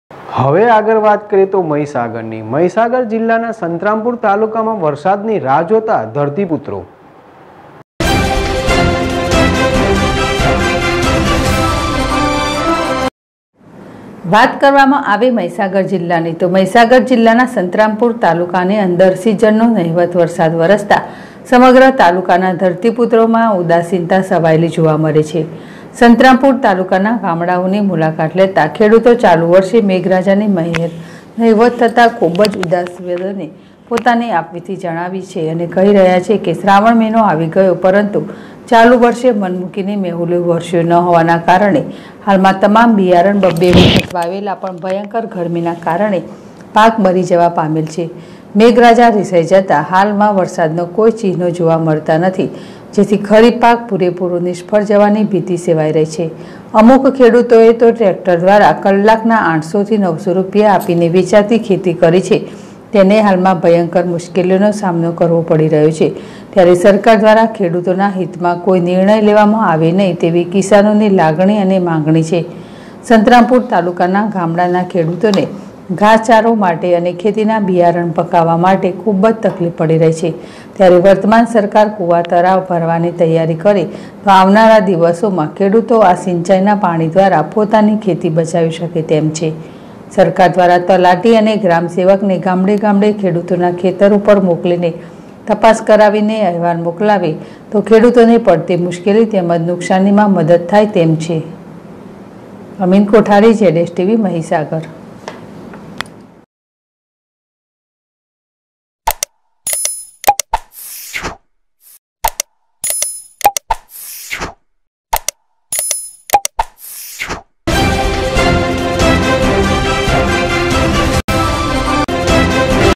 સંતરામપુર તાલુકા ની અંદર સિઝનનો નહિવત વરસાદ વરસતા સમગ્ર તાલુકાના ધરતીપુત્રો મા ઉદાસીનતા છવાયેલી
સંતરામપુર તાલુકા ની અંદર સિઝનનો નહિવત વરસાદ વરસતા સમગ્ર તાલુકાના ધરતીપુત્રો મા ઉદાસીનતા છવાયેલી જોવા મળે છે.